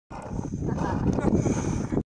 23 Nov 2012 - Valley of the Moon(walk) - Atacama Desert - Chile
- Moonwalking in the Valley of the Moon - Atacama Desert. Moonwalking in hiking boots on rock is apparently a hell of a lot harder than I anticipated.